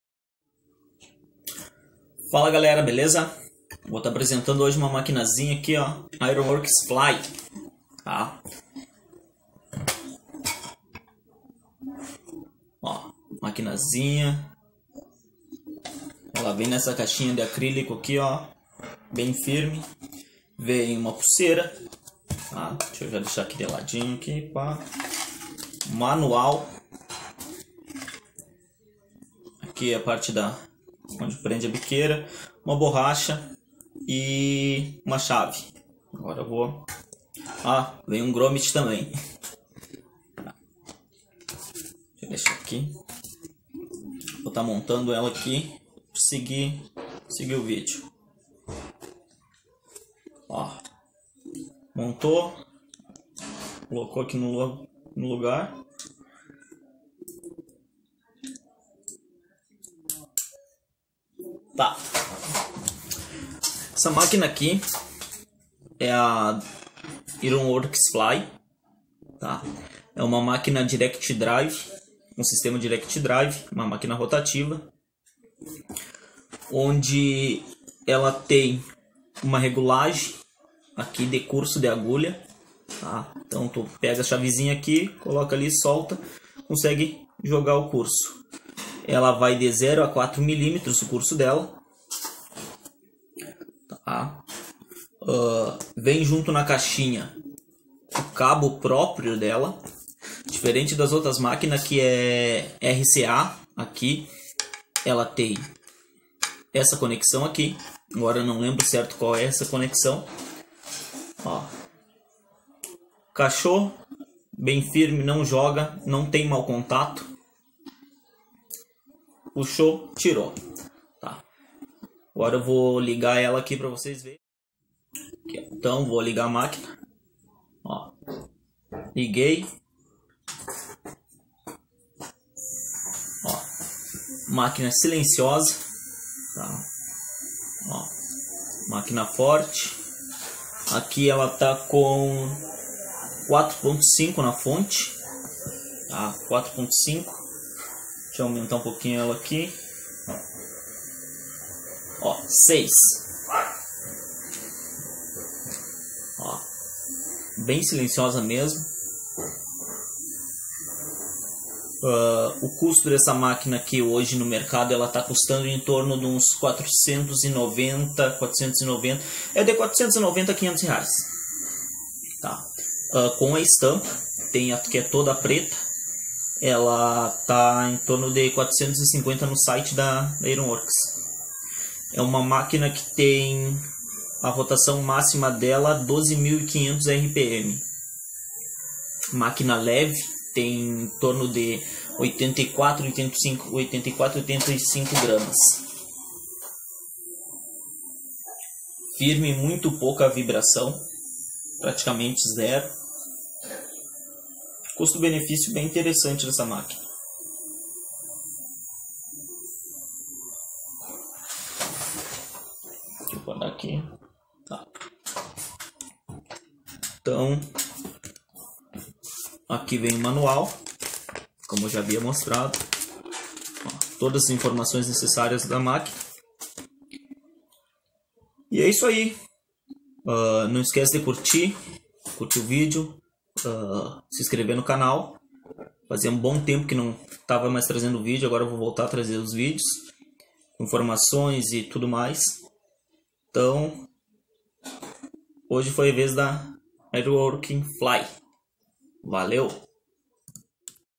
apresentação máquina Iron works fly por Robson Reis
ironworksfly a máquina rotativa da iron works brasil